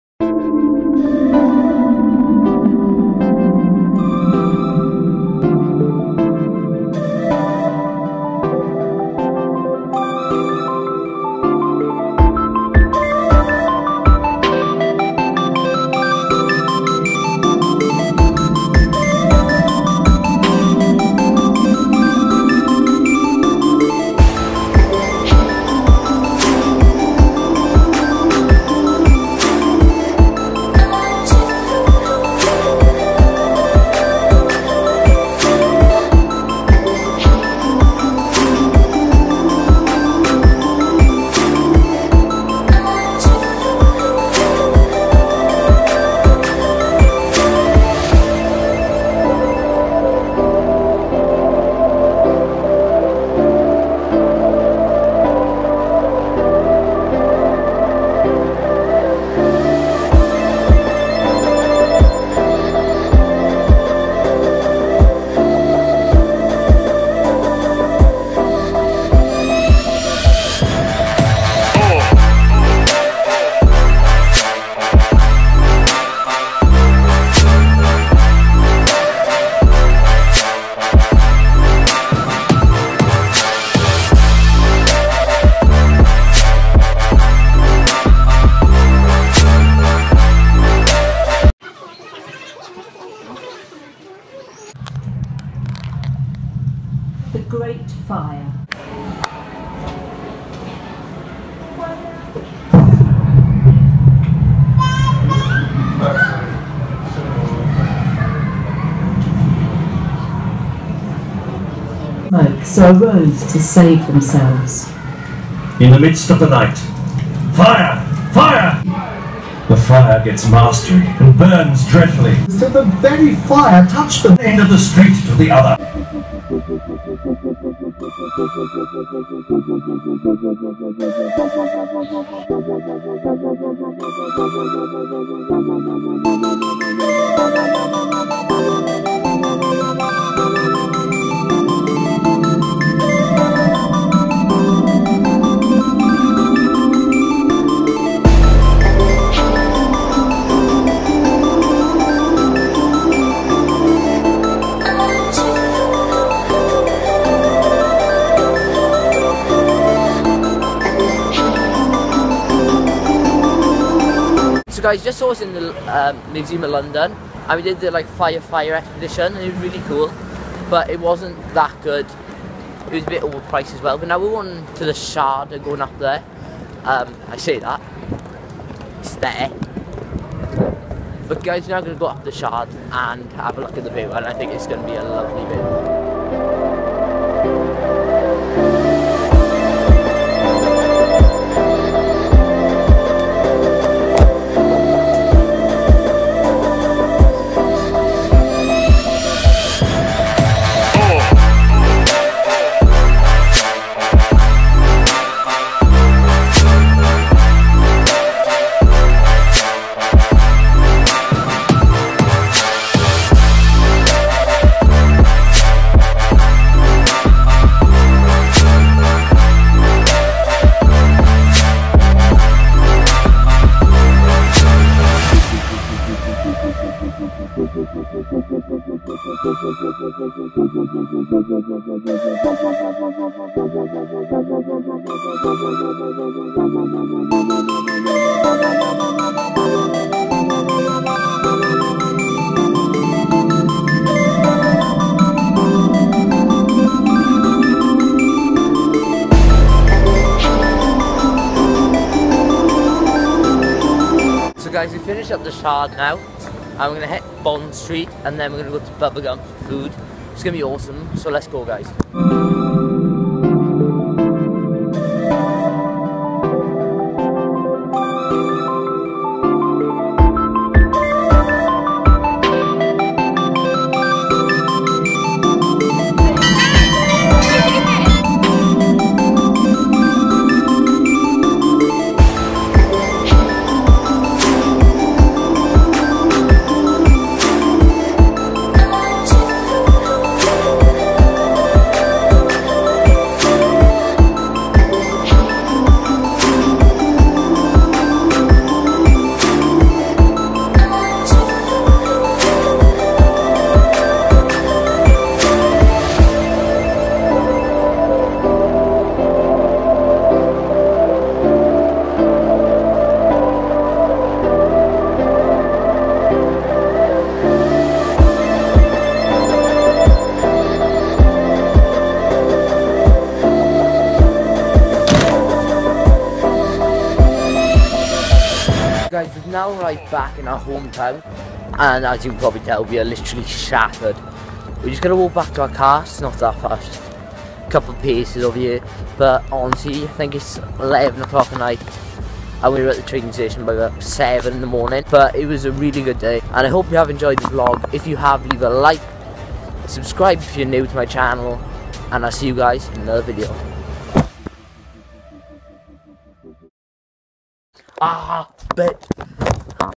TOP OF THE SHARD?!
Welcome to another vlog. In today's vlog we go to the top of the Shard, go into the Museum of London to see the new 'Fire Fire' expedition. I hope you enjoy the video.